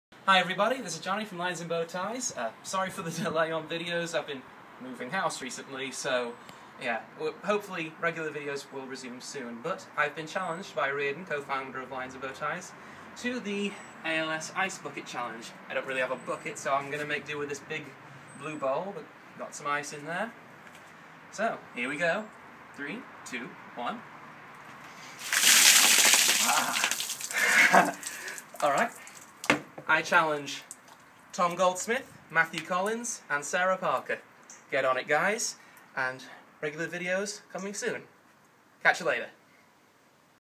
ALS ice bucket challenge - Johnny from Lions in Bowties
Riordan challenged me, I accepted :D